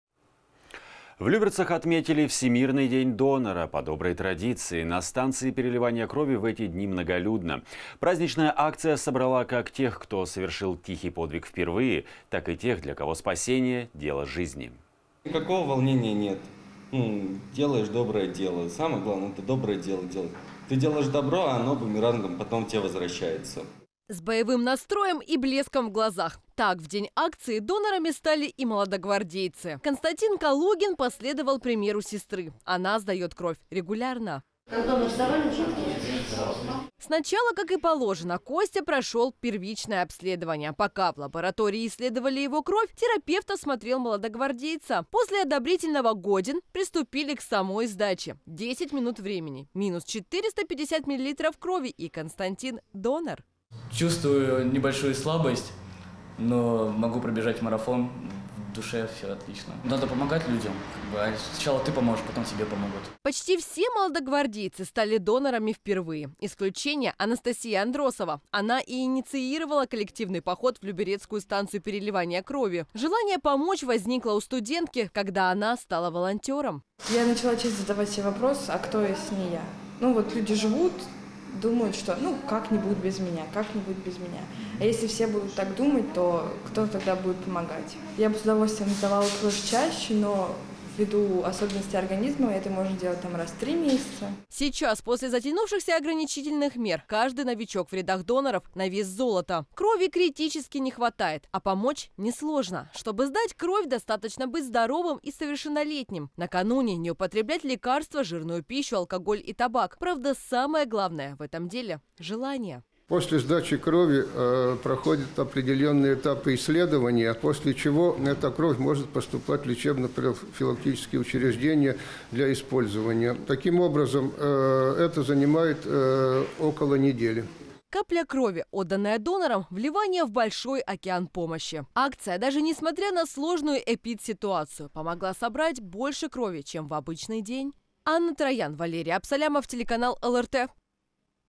«Тихий подвиг» «Молодой гвардии»
В Люберцах отметили Всемирный День донора. По доброй традиции на станции переливания крови в эти дни многолюдно. Праздничная акция собрала, как тех, кто совершил «тихий подвиг» впервые, так и тех, для кого спасение — дело жизни.

Глеб Шмидт, донор:
"Никакого волнения нет. Делаешь доброе дело. Это самое главное. Делаешь добро, а оно тебе возвращается бумерангом.."

С боевым настроем и блеском в глазах. Так в день акции донорами стали и молодогвардейцы. Константин Калугин последовал примеру сестры, она сдает кровь регулярно.

Сначала, как и положено, Костя прошел первичное обследование. Пока в лаборатории исследовали его кровь, терапевт осмотрел молодогвардеца. После одобрительного «годен» приступили к самой сдаче. 10 минут времени, минус 450 мл крови и Константин донор.

Константин Калугин, волонтер:
"Чувствую слабость, но могу пробежать марафон. В душе всё отлично. Надо помогать людям. Сначала ты поможешь, потом тебе помогут.."

Почти все молодогвардейцы стали донорами впервые. Исключение – Анастасия Андросова. Она и инициировала коллективный поход в люберецкую станцию переливания крови. Желание помочь возникло у студентки, когда она стала волонтером.

Анастасия Андросова, донор:
"Я начала чаще задавать себе вопрос «Кто, если не я?». Люди живут, думают «ну, как-нибудь без меня». А если все так будут думать? Кто тогда будет помогать?Я с удовольствием бы сдавала кровь чаще, но ввиду особенностей организма, это можно делать раз в три месяца.."

Сейчас, после затянувшихся ограничительных мер, каждый новичок в рядах доноров – на вес золота. Крови критически не хватает. А помочь не сложно: чтобы сдать кровь, достаточно быть здоровым и совершеннолетним. Накануне не употреблять лекарства, жирную пищу, алкоголь и табак. Правда, самое главное, в этом деле –желание.

Владислав Судечкис, И.О. заведующего люберецкой станцией переливания крови:
"После сдачи крови проходят этапы исследования, после чего эта кровь может поступать в лечебно-профилактические учреждения для использования.Таким образом, это занимает около недели.."

Капля крови, отданная донором – вливание в большой океан помощи. Акция, даже несмотря на сложную эпидситуацию, помогла собрать больше крови, чем в обычный день.